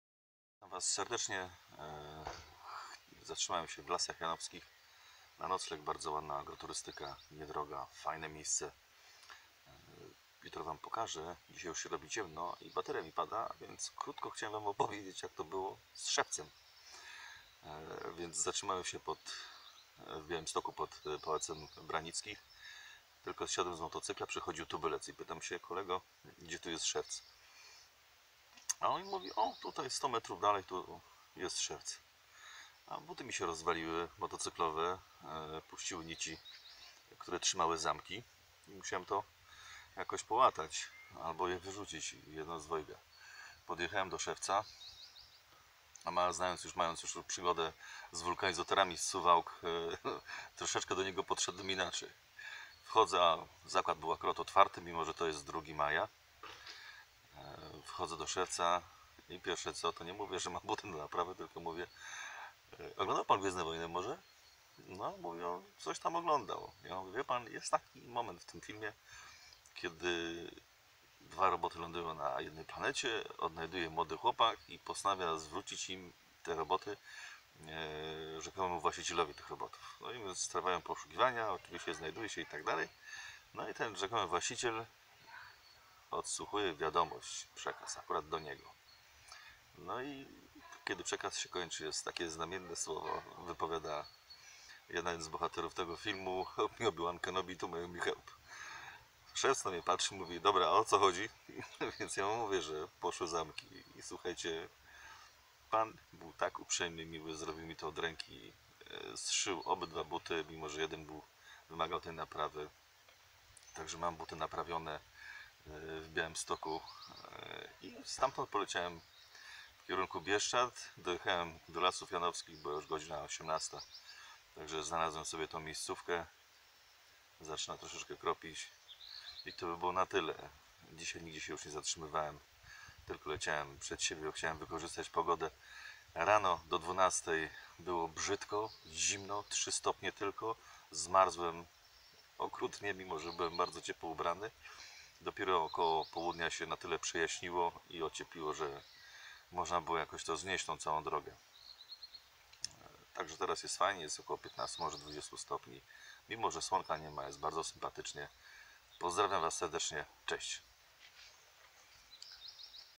Lasy Janowskie - czyli jak to z butami było
Trzecia relacja z wyjazdu w Bieszczady Junakiem RS125 Pro